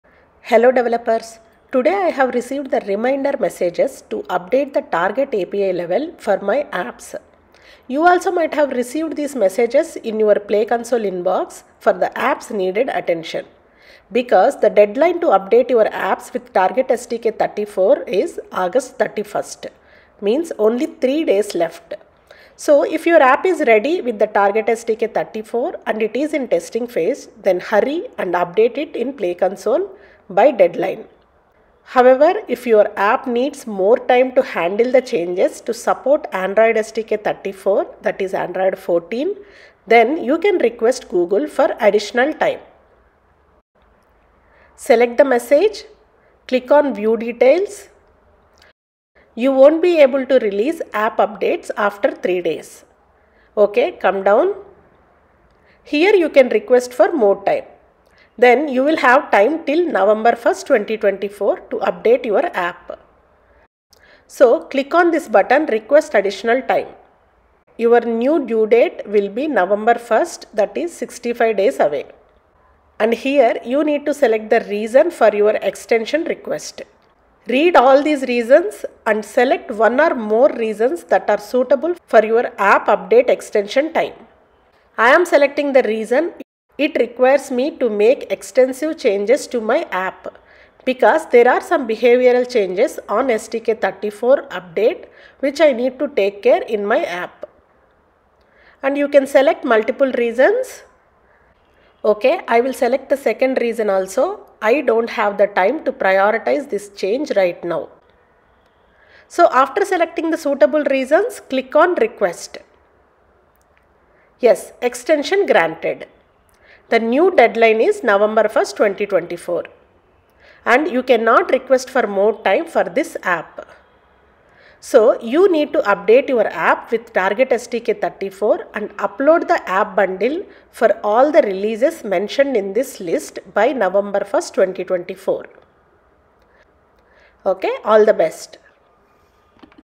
⚠️ Extend the deadline for the app update with target API level 34 | Aug 31 2024 to Nov 1 2024
If you need more time to update your app with target api level 34, then "Request additional time" from google play. The deadline will be extended from August 31 2024 to November 1 2024.

Fix for "Update your target API level by August 31, 2024 to release updates to your app"